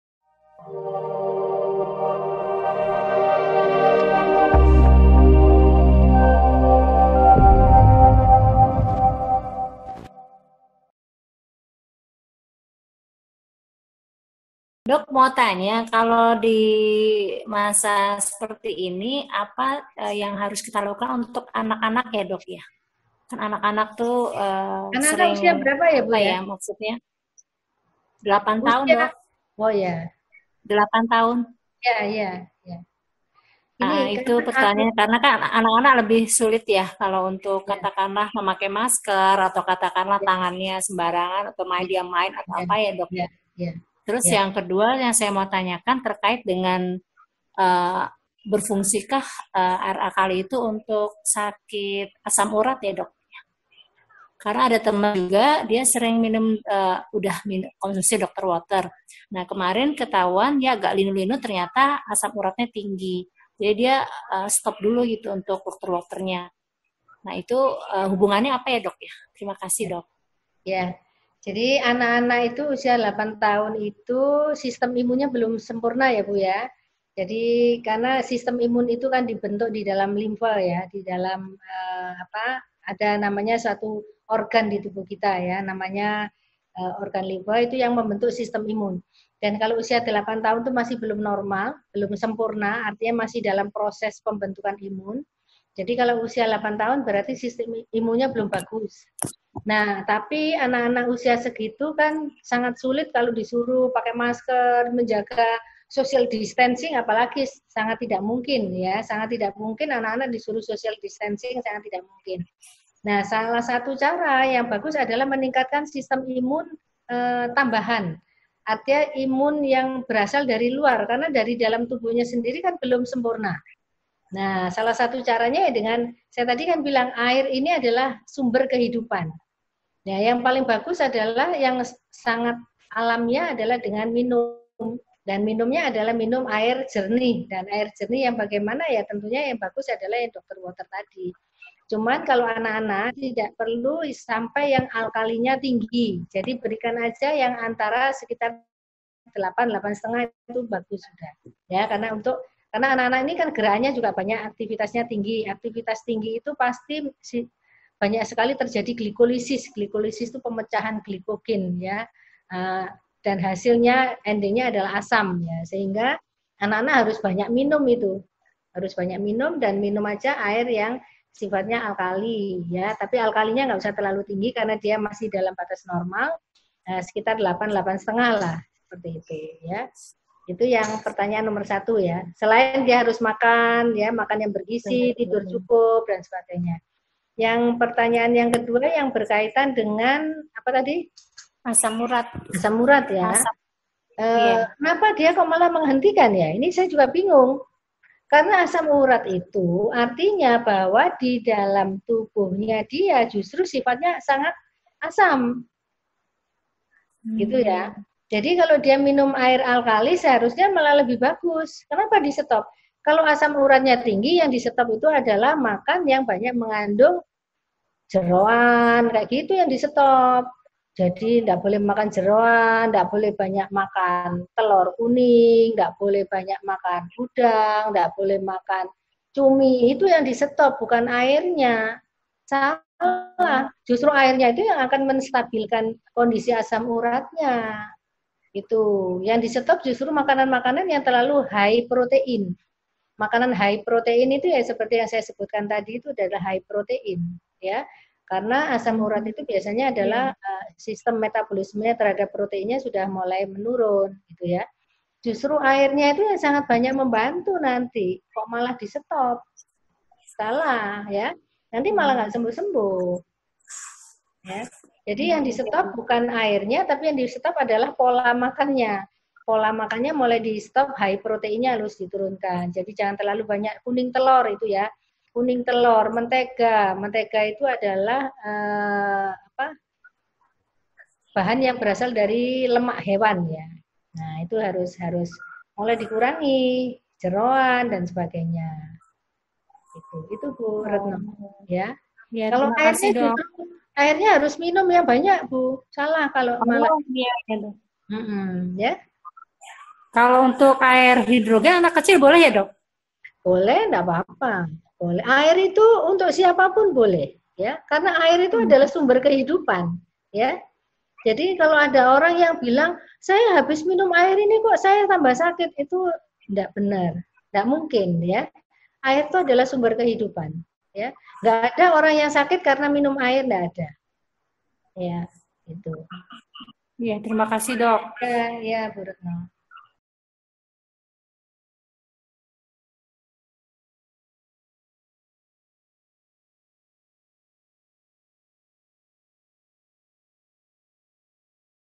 Apakah Orang Penyakit Asam Urat Boleh Mengkonsumsi Air Alkaline ? | Q n A Produk Altrams
Yuk simak penjelasan tentang Apakah Orang Penyakit Asam Urat Boleh Mengkonsumsi Air Alkaline ? Bersama Dr. Theresia Indah Budhy

For bussines :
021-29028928